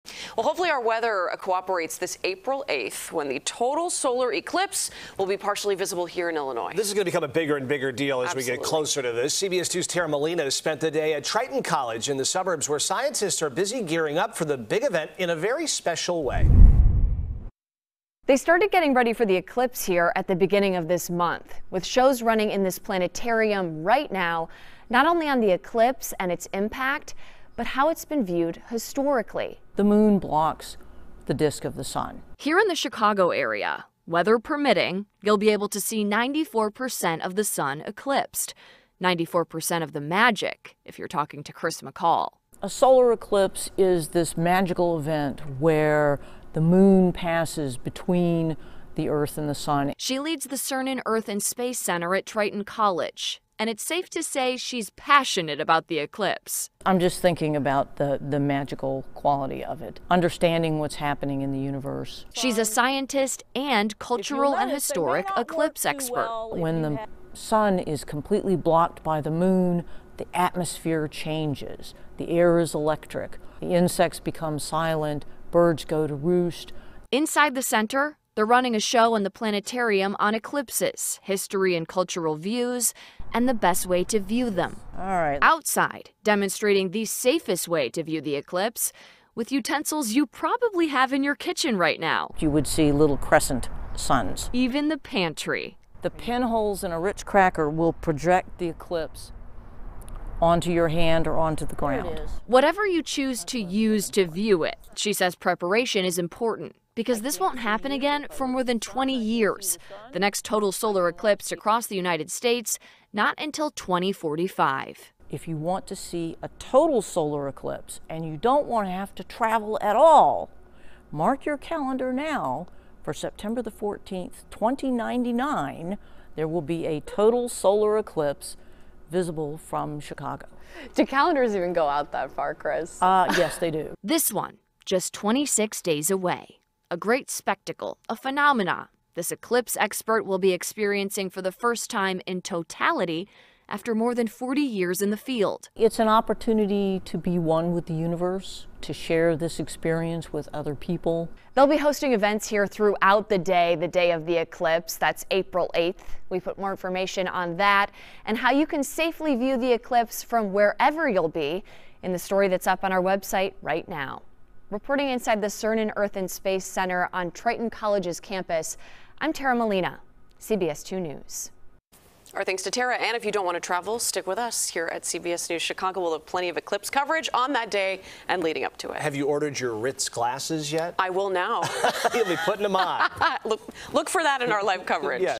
Excitement builds for total solar eclipse in April
In less than a month, a solar eclipse will darken the skies over the Chicago area, and at Triton College in west suburban River Grove, scientists were busy Tuesday gearing up for the big event in a special way.